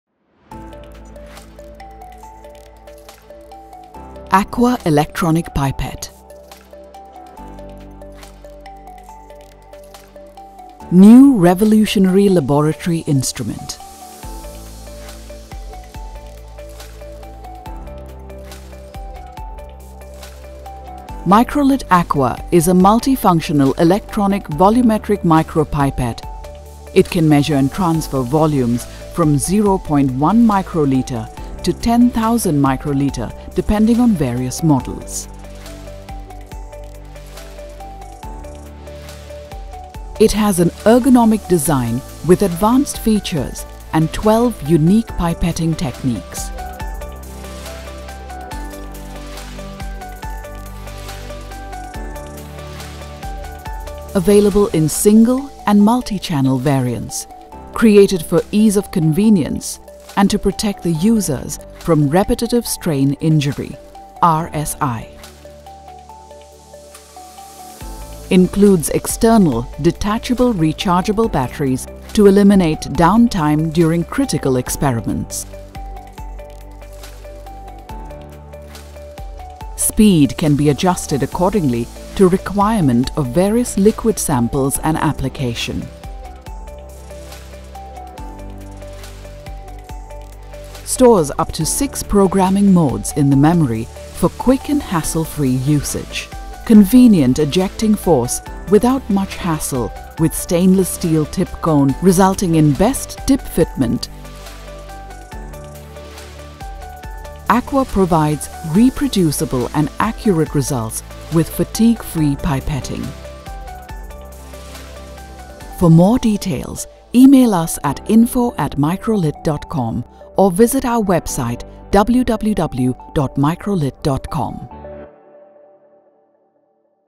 AQUA Multi-functional Electronic Pipette
AQUA provides reproducible and accurate results with fatigue-free pipetting.

Speeds up Workflows

Multiple Pipetting Modes

Saves Time and Effort

Improves Traceability

Avoid Human Errors